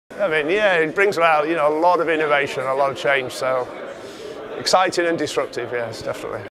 Internet Future in a Nutshell? IETF 2015: Andrew Hutton
Andrew Hutton, Head of Standardization at Unify, shares his answer to the question “Using just a few words, in just five seconds or less, describe the future of the Internet.” at Internet Engineering Task Force meeting 92, which took place in March 2015 in Dallas, Texas. Recorded by the Imagining the Internet Center, an initiative of Elon University, North Carolina, USA.